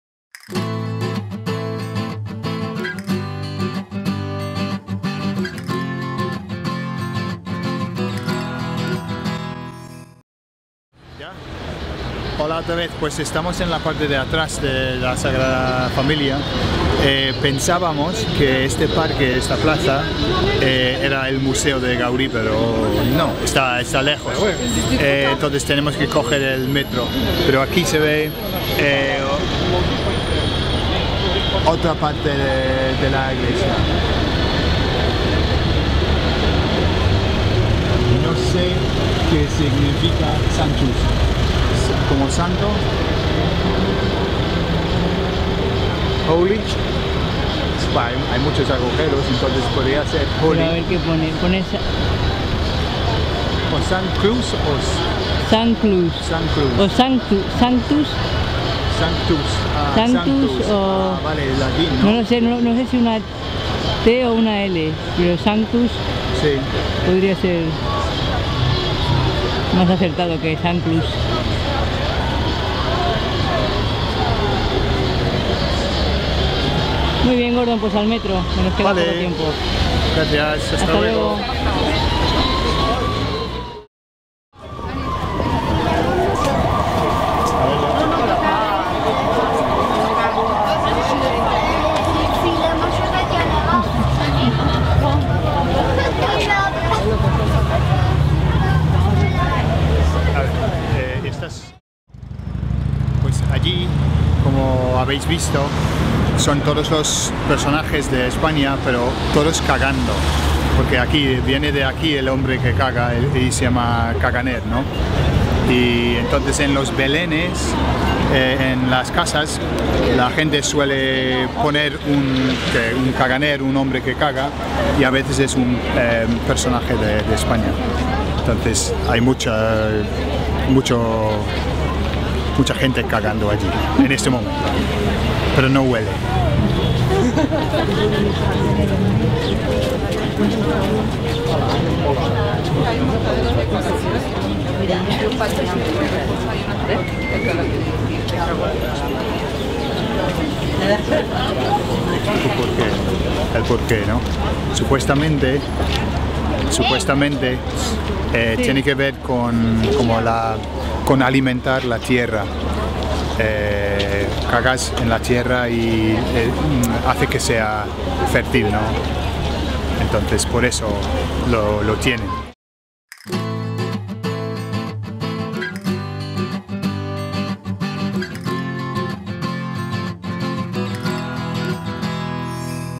The Man who Poos at Christmas Our Weekend Away LightSpeed Spanish
we are pleased to offer you a insight into our weekend away to Barcelona to see Monica Naranjo in concert.